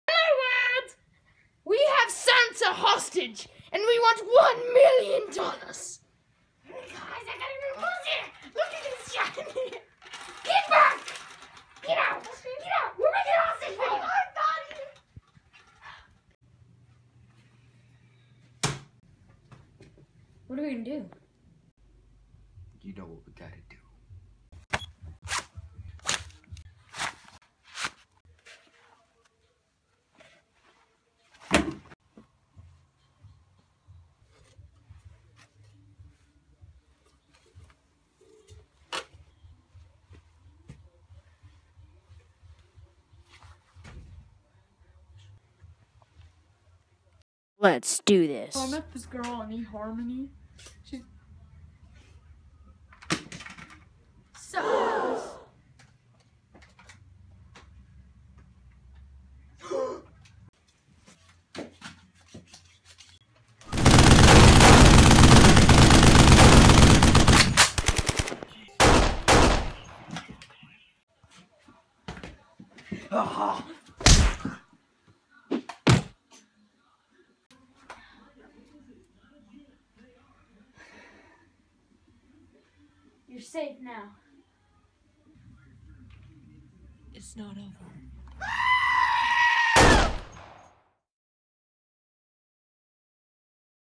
Saving Santa
As you can this isn't the original film group.  There is Matt, John, Avery, Hunter, and Jo.  Santa was unharmed in this video.  The rescue mission failed at first. But later on the CIA came and saved Christmas.